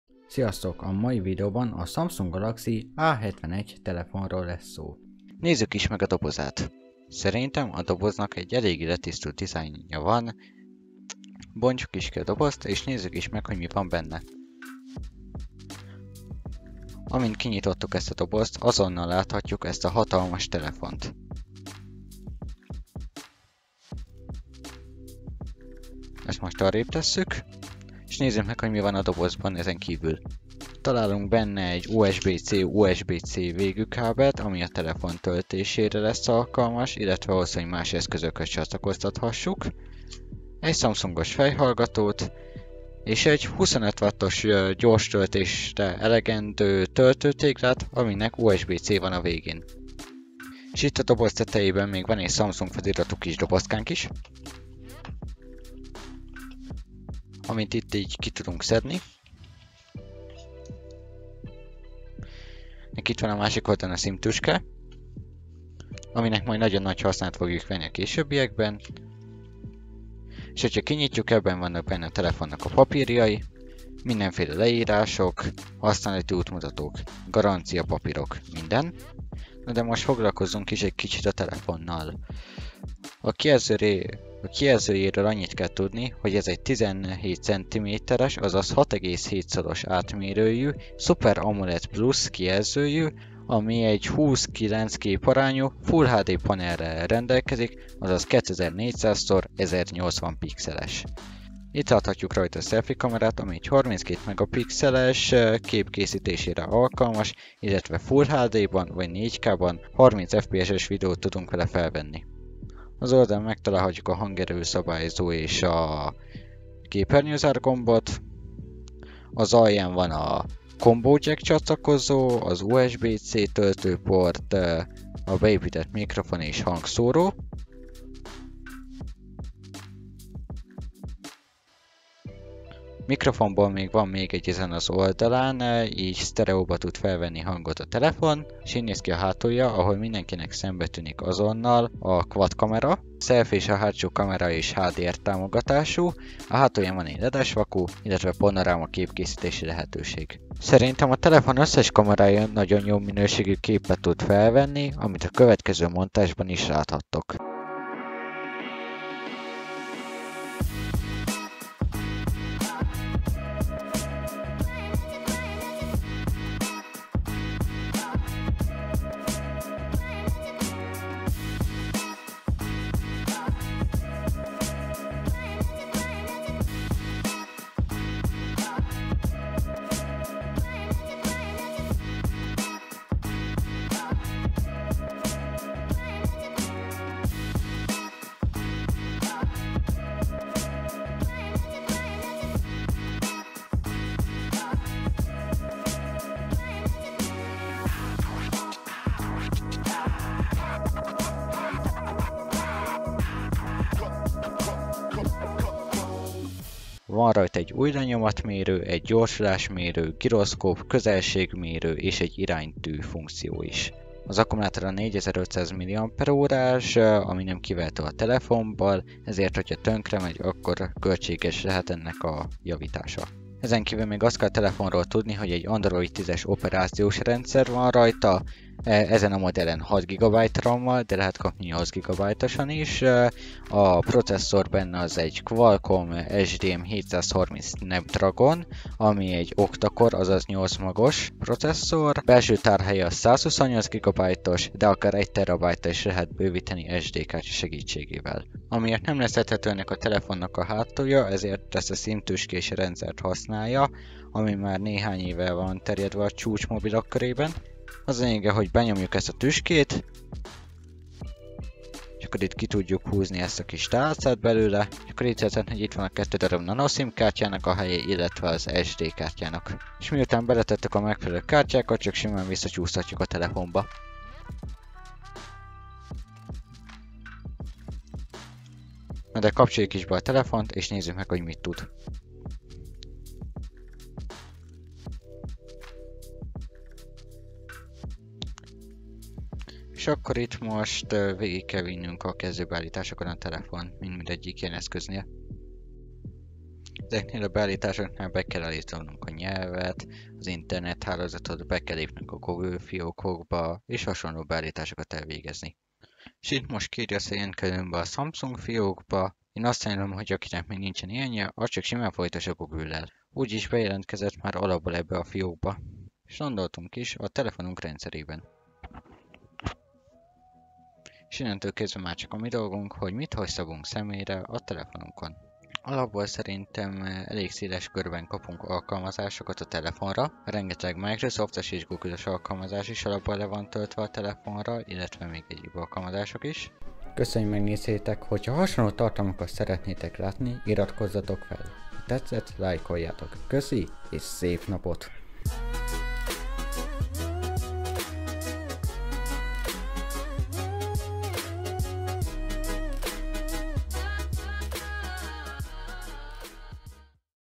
Samsung Galaxy A71 Review
Ez a videó NEM szponzoráció segítségével jött létre.
0:00 - Bevezetés
0:05 - Unboxing
1:19 - Specifikáció
1:52 - Oldalai, hátulja
2:19 - Kamera minősége
2:36 - Kamera montage
3:26 - Szenzorok
3:33 - Akkumlátor
3:43 - Alkatrészek
4:08 - Kártyák behelyezése
4:38 - Beüzemelés
5:36 - Végszó
Kamera: HP V5560u